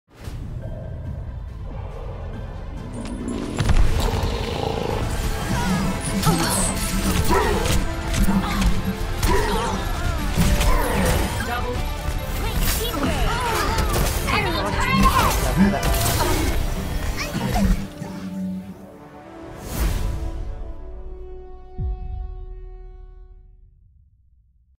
ǁpxntherǁ's highlight 18 09 02 12 10 32
Was a team kill with the reaper and hammond dying after :D